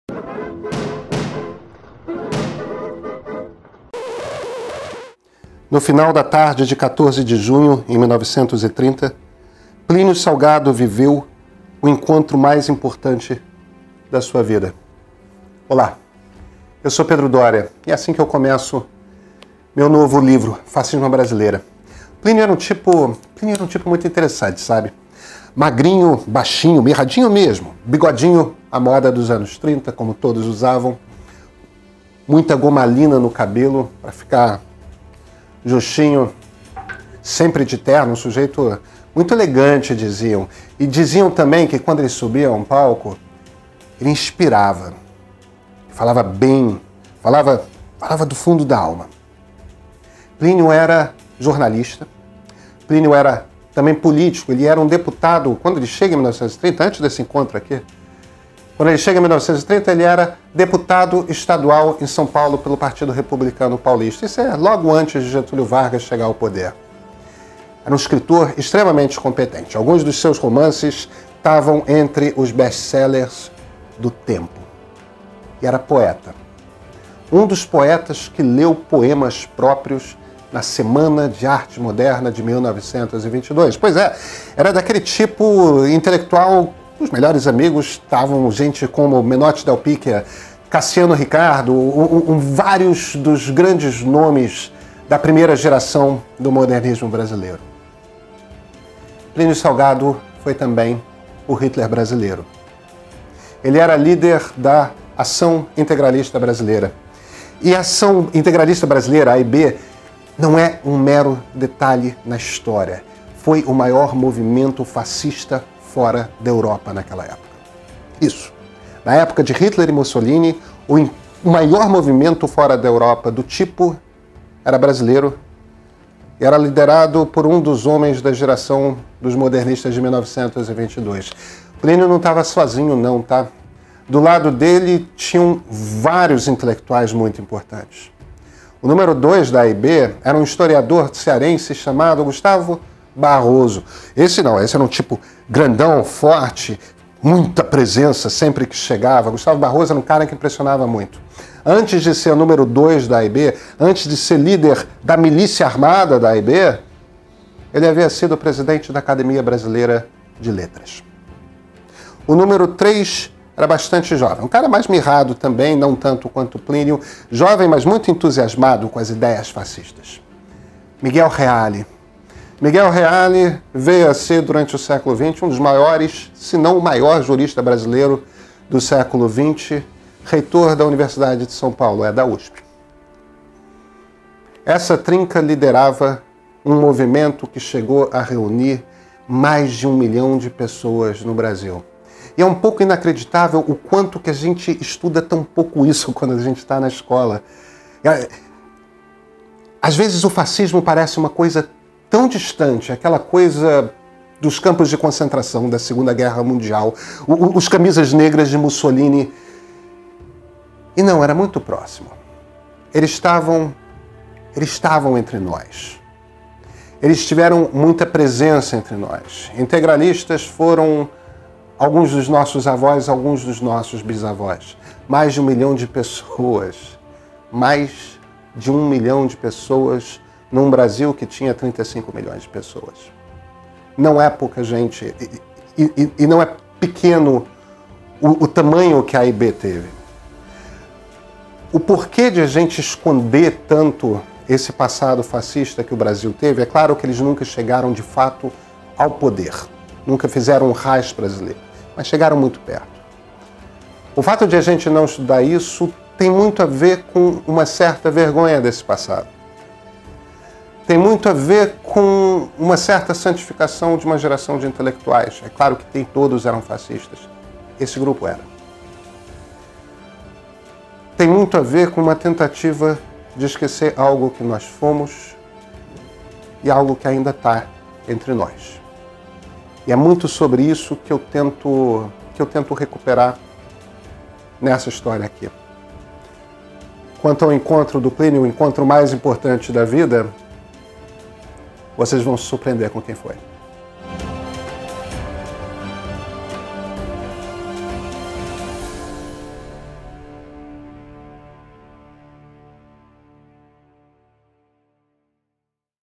Quem foi Plínio Salgado? - Fascismo à brasileira
"No final da tarde de 14 de junho de 1930, Plínio Salgado
viveu o encontro mais importante de sua vida..."

Se é inegável que entende melhor o presente quem conhece o passado, é fundamental conhecer o integralismo para compreender a essência do bolsonarismo. Sim, o Brasil teve um movimento fascista e anticomunista na sua história na mesma época de Mussolini e Hitler. Fundado pelo deputado e jornalista Plínio Salgado, a Ação Integralista Brasileira foi o maior movimento fascista fora da Europa entre os anos 1920 e 1940 – e também o maior movimento de extrema-direita no país.